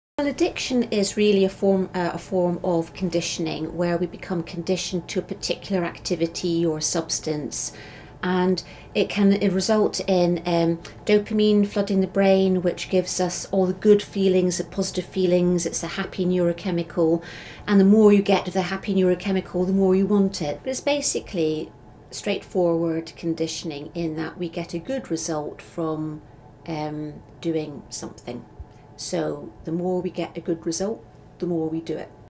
SCIENCE WEB
It's addiction week here at UoGlos Live, but how do we become addicted?